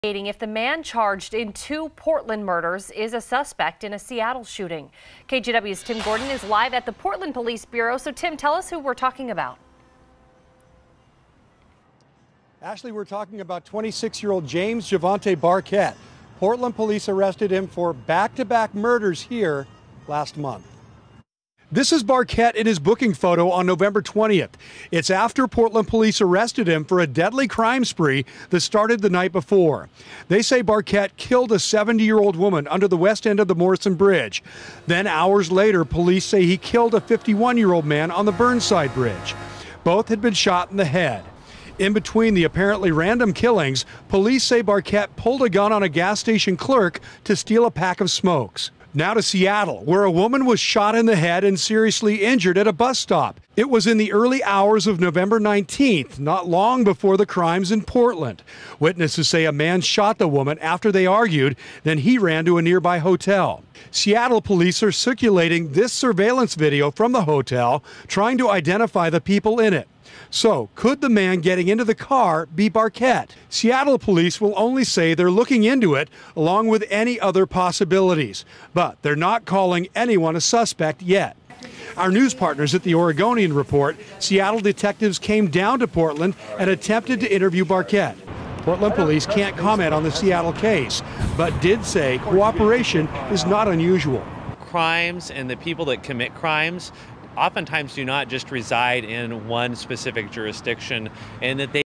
Seattle police looking at Portland double-murder suspect
Seattle detectives are investigating whether Javantae Barquet, accused of two stranger-to-stranger shooting deaths in downtown Portland, may be linked homicides there.